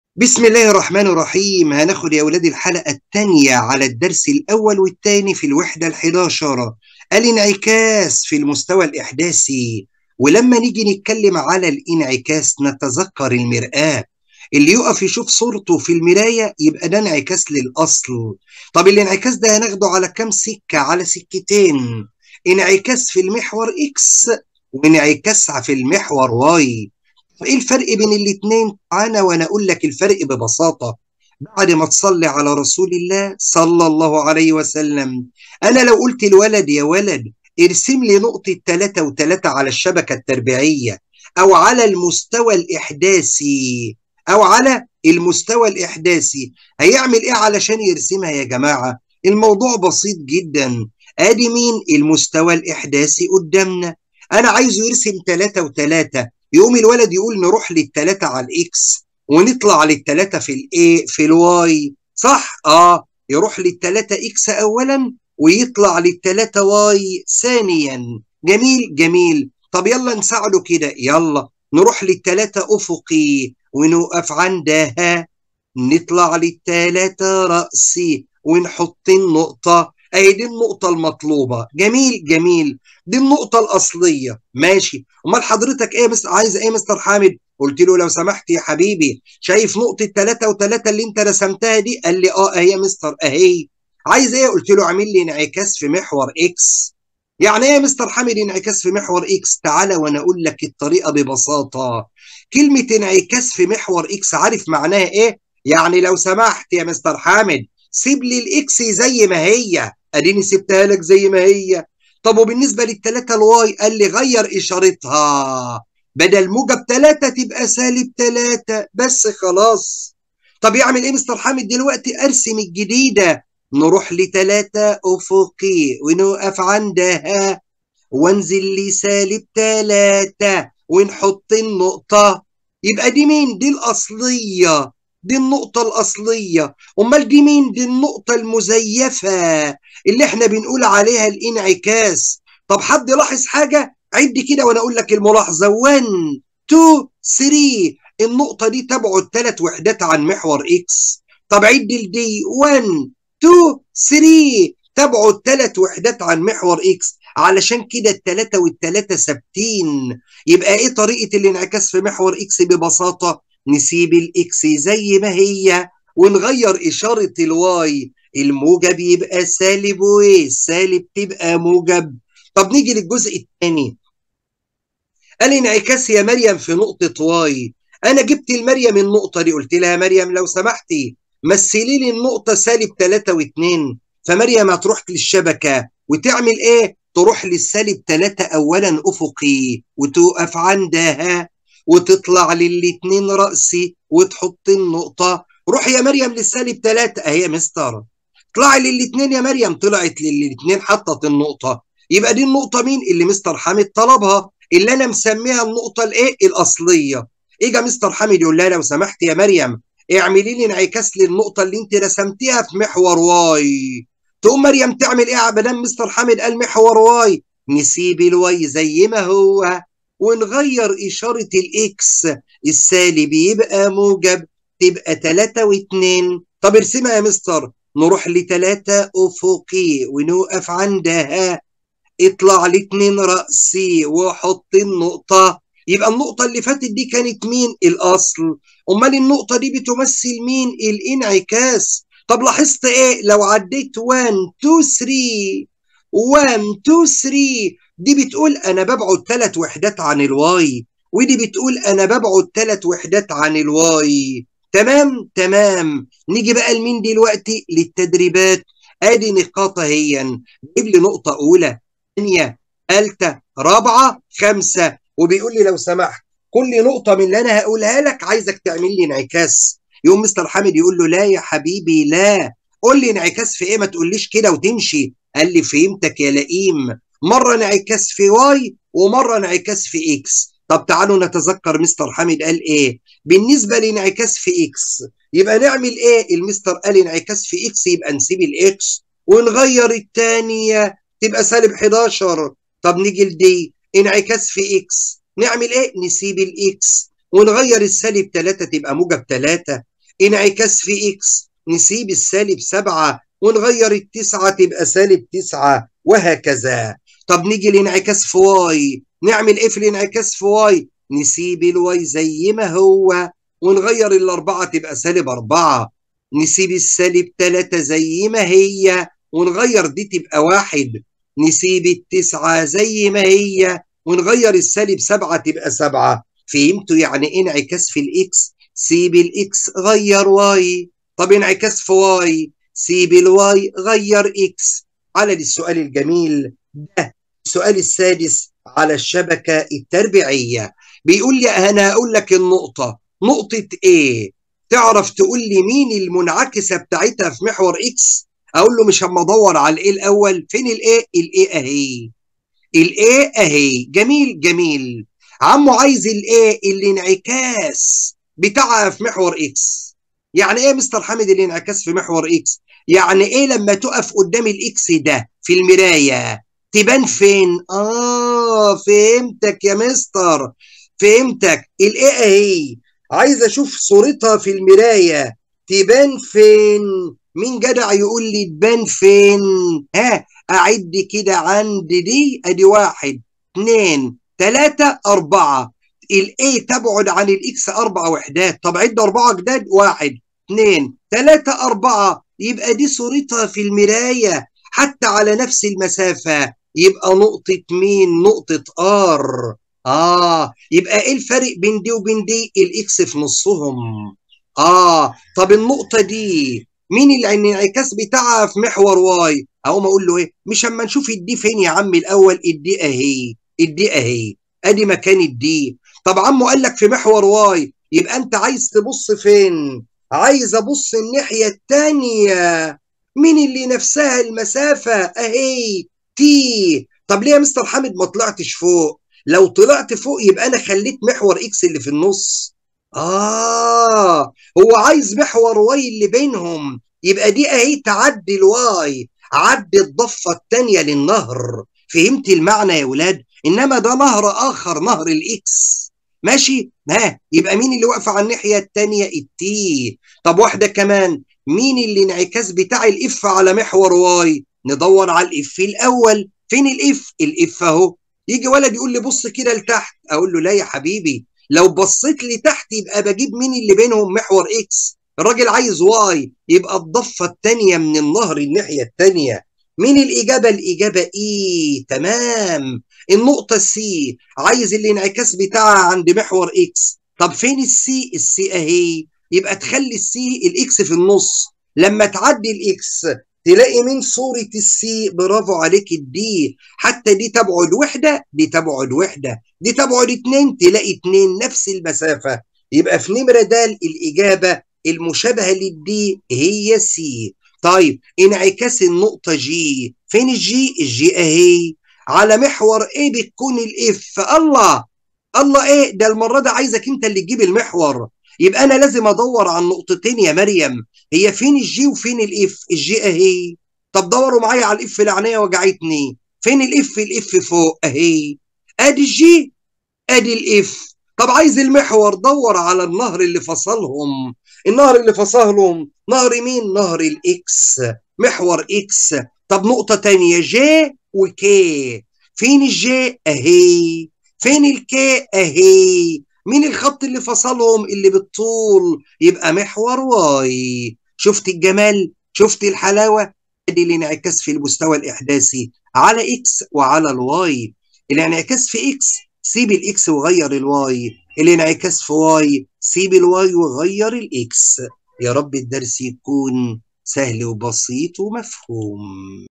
الإنعكاس في المستوي الاحداثي الدرس الثانى الوحدة الحادية عشرة رياضيات الصف السادس مستر حامد البيومى
الإنعكاس في المستوي الاحداثي الدرس الثانى الوحدة الحادية عشرة رياضيات الصف السادس مستر حامد البيومى بسهولة وبساطة وشرح وافي مفصل مع تدريبات متنوعة عامة هامة شاملة علي قناة روائع البيومى الوحدة 11 الترم الثانى 2024
مع مراجعات واختبارات ومقرر امتحان شهر مارس